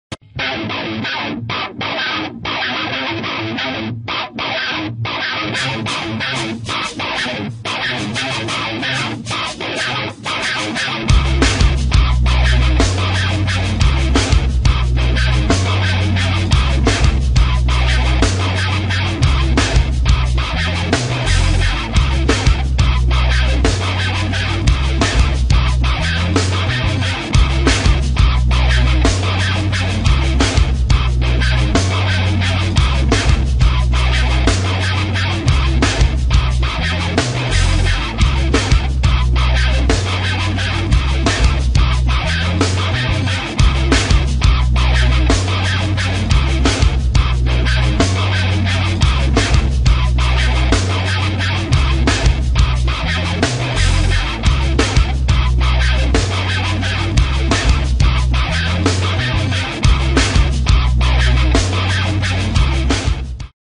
my new new intro :D
yeah i thought i give it another shot and here it is
that image took 3 minutes to make and then i put the clip speed to 300% in camstudio  so  stfu if u think its a bad picture for 3 mins work